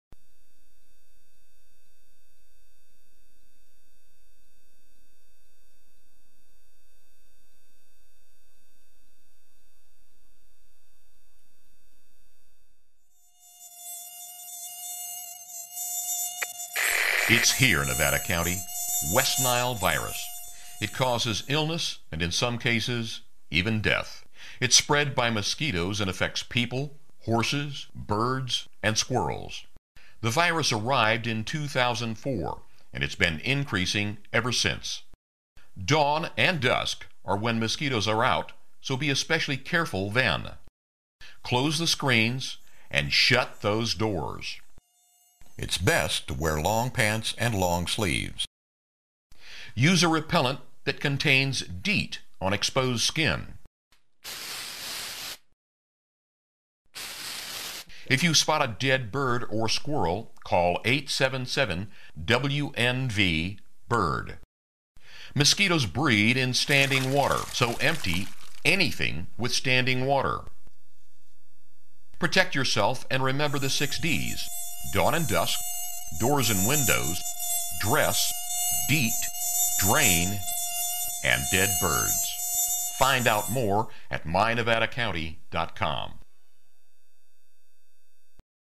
West Nile PSA Program
This is 90 sec. PSA created for the County Of Nevada, California.  It shows how to protect yourself again the West Nile Virus.